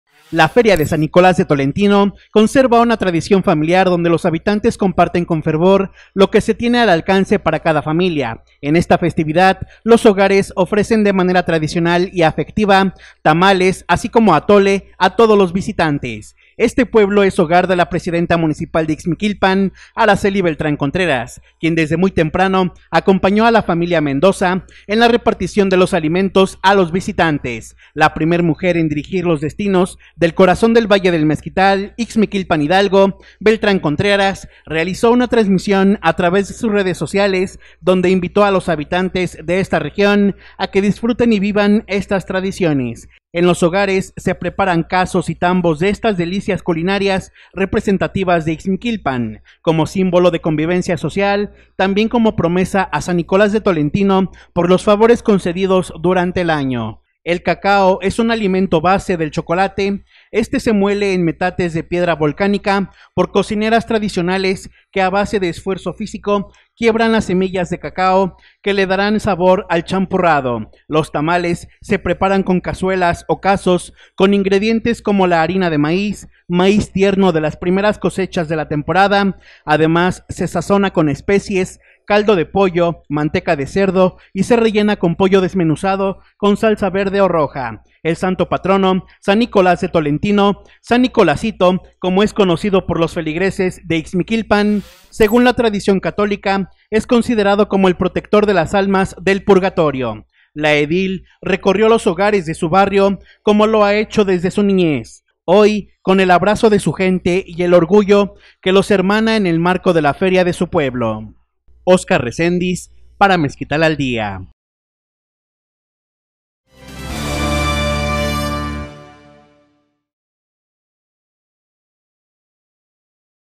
Una tradición familiar, la Feria de San Nicolás en el municipio de Ixmiquilpan
Familias enteras se reúnen para disfrutar del delicioso champurrado y los tamales tradicionales durante los festejos en honor a San Nicolás de Tolentino en el municipio de Ixmiquilpan.